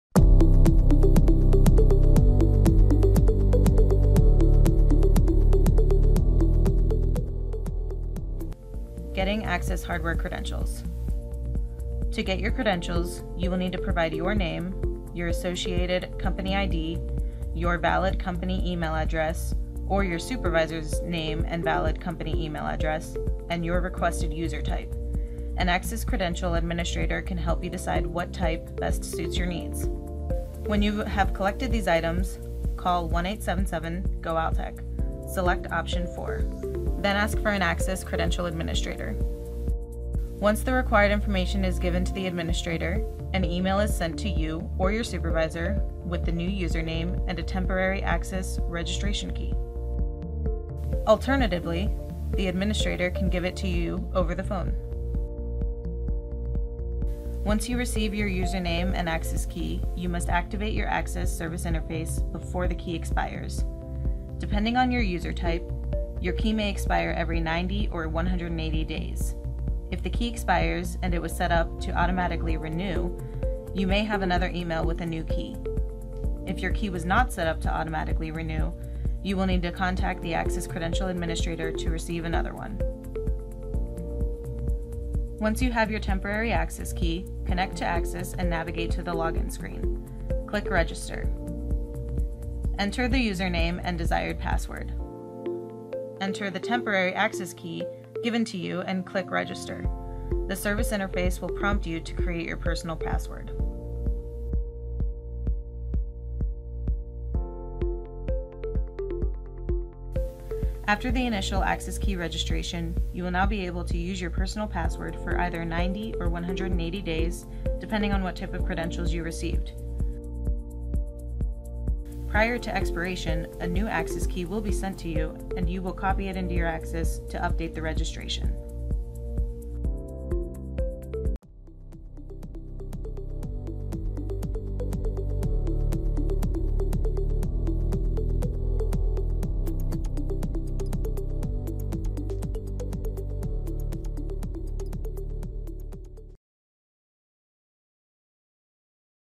Altec Axis Service Interface
Find out how to get Axis Hardware Credentials here.

The AXIS Service Interface is Altec’s preferred service tool. It’s rich user interface and intuitive system allows for quick and easy calibration and diagnosis of Altec machines.The AXIS interface leverages the benefits of a mobile device to provide an instinctive platform to work from. AXIS enables a Wi-Fi connection which allows the user to move freely and safely around the machine without the hassle of a tether. The device also supports remote firmware downloads so the user can easily take advantage of updated features and supported Altec equipment.